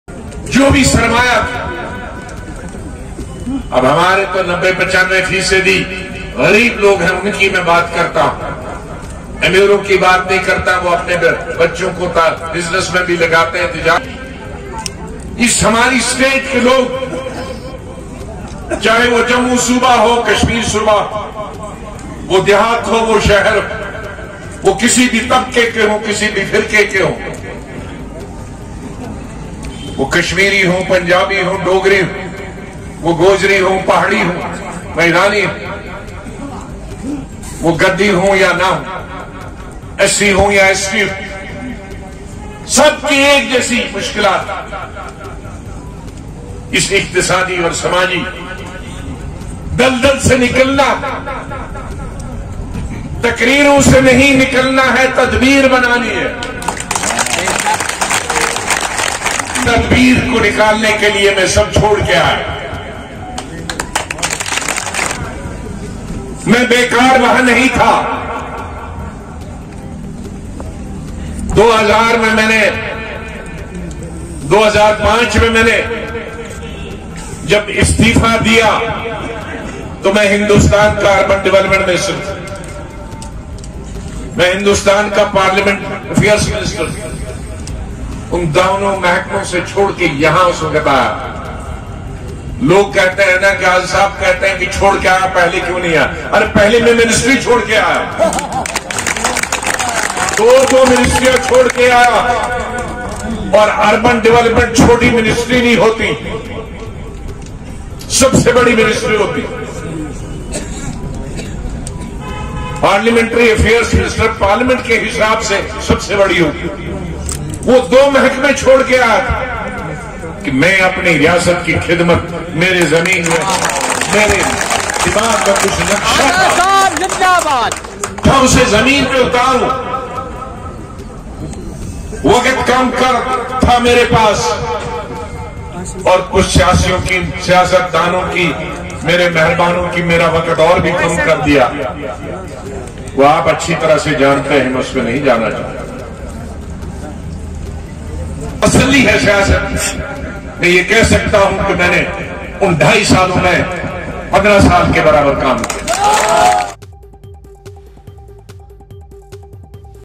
DPAP celebrated first foundation day, Azad Dispels ‘Rumours’ About Being Next LG Of JKUT
DPAP celebrated its first foundation day in Srinagar, Azad Dispels ‘Rumours’ About Being Next LG Of Jammu And Kashmir

DPAP के स्थापना दिवस पर गुलाम नबी आजाद ने पार्टी कार्यकर्ताओं को किया संबोधित, जम्मू-कश्मीर का अगला उपराज्यपाल बनने की 'अफवाहों' को किया खारिज